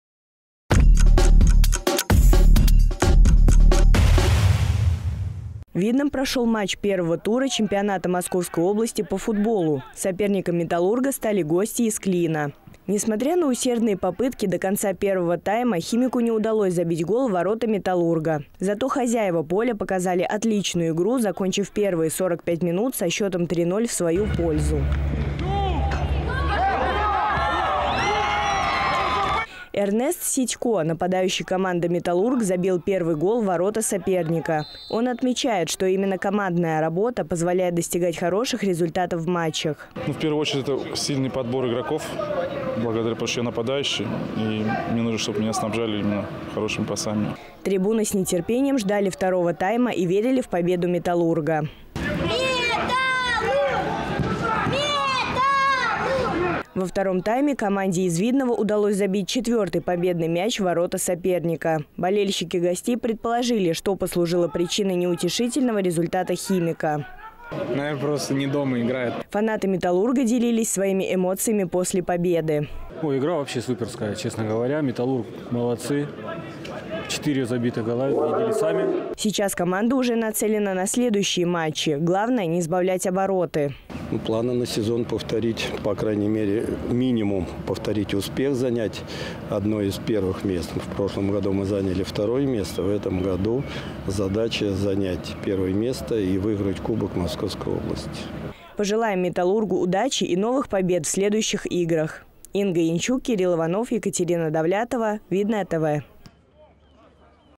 «МЕТАЛЛУРГ» - «ХИМИК»
В первом туре чемпионата Московской области по футболу Видновский Металлург встретился на своём поле с Клинским Химиком. За ходом игры следила Инга Янчук